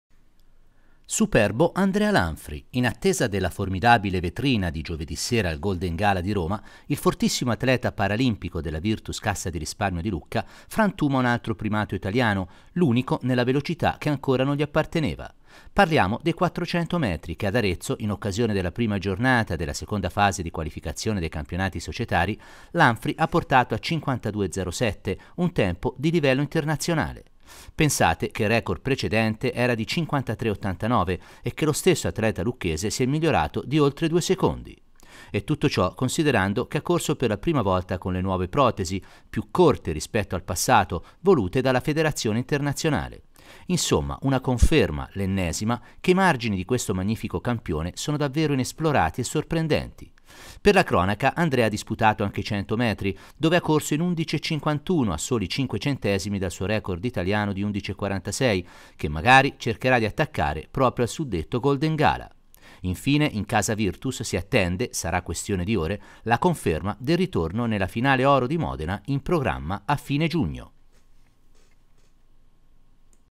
Andrea Lanfri frantuma il record italiano sui 400
ATLETICA - Un grandissimo Andrea Lanfri migliora il record italiano dei 400 metri paralimpici categoria T62 con il tempo di 52"07. Il forte atleta della Virtus CR Lucca era alla prima gara con le nuove protesi. Buon riscontro anche sui 100 (11"51) in attesa della gara di giovedì sera al Golden Gala di Roma.---

NoiTv